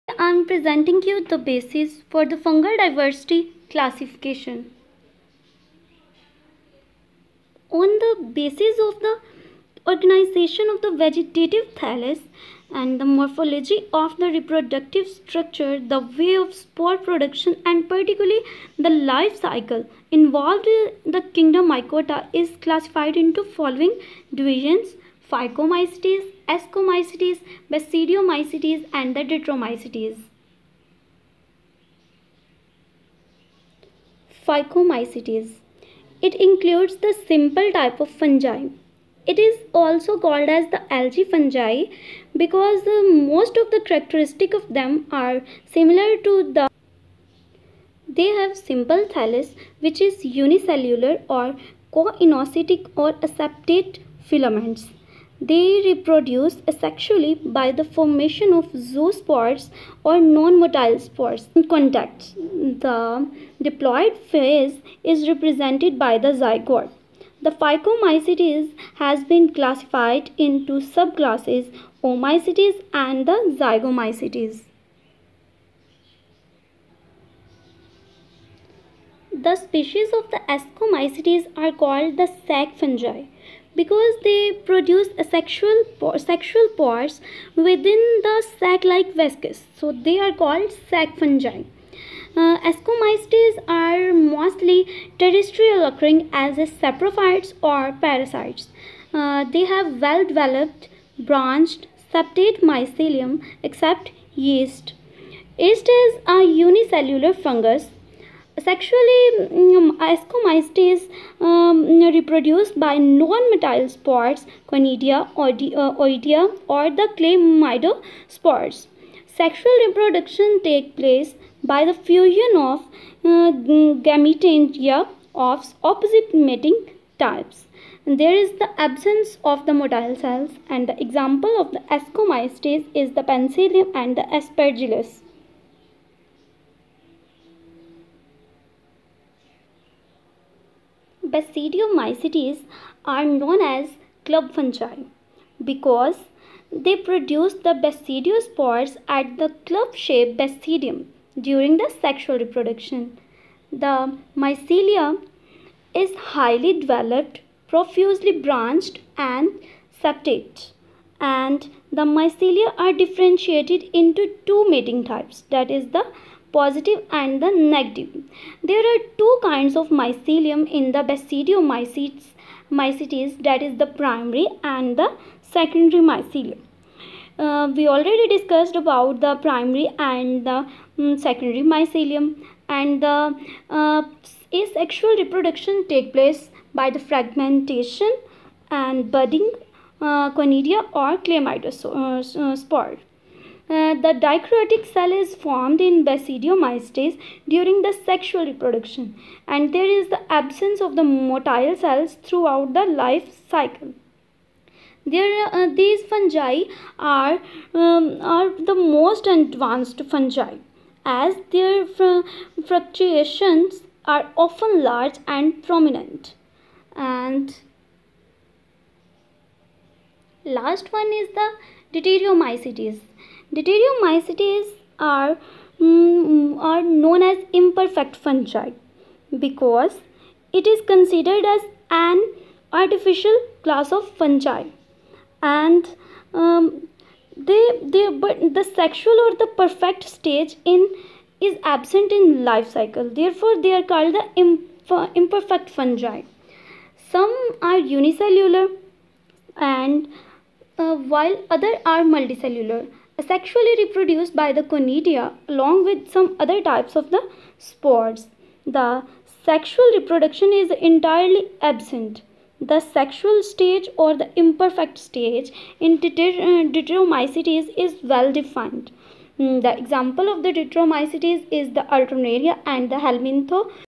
Classification of fungi ( Part-3 ) Based on the Spore Production
On the Basis of Spore production fungi is classified into ;
1. phycomycetes
2. Ascomycetes
3. Basidiomycetes
4. Deuteromycetes

for the explanation of such terms watch the video.

Thanks!